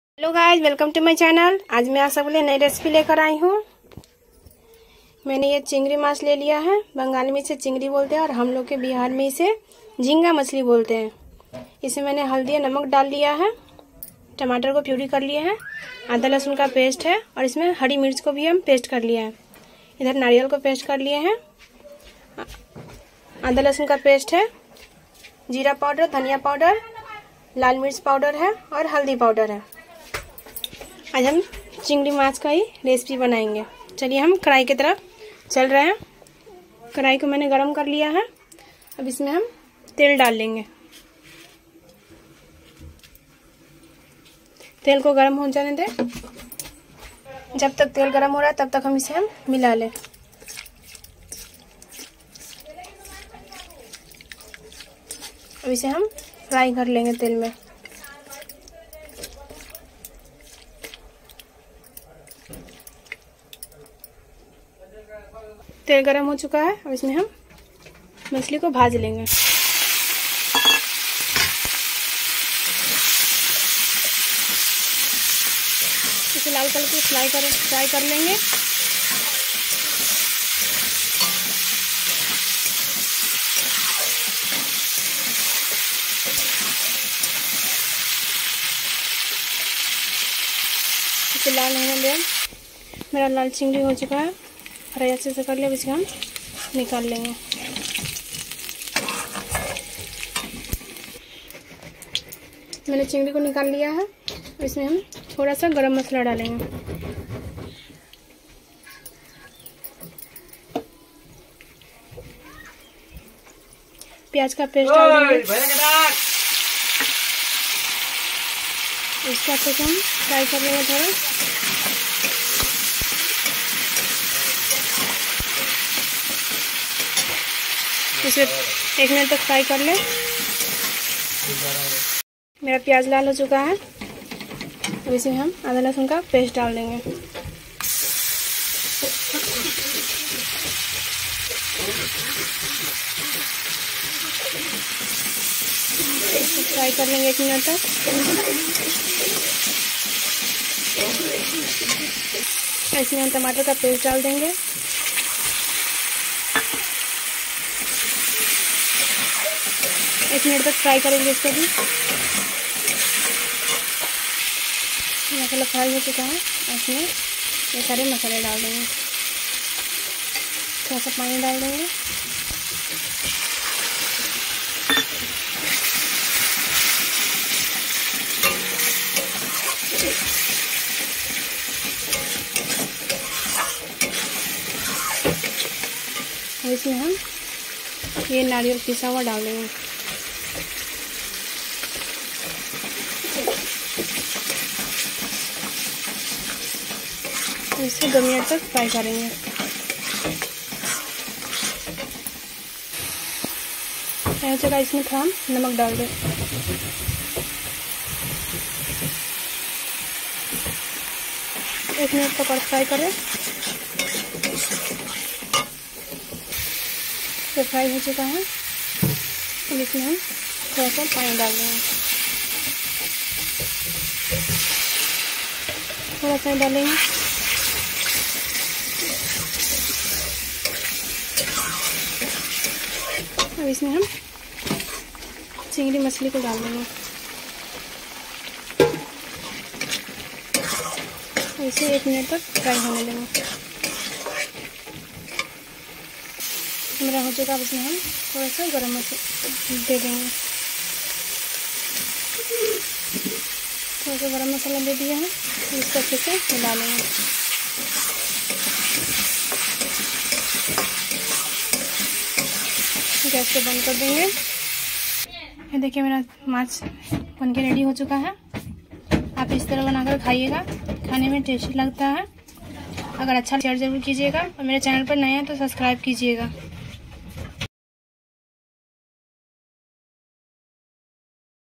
chingri Mach (jhinga machli) nariyal dekar banye es tarike se to sbhi tarif karenge kajal cooking
chingri Mach (jhinga machli) nariyal dekar banye es tarike se to sbhi mehman tarif karenge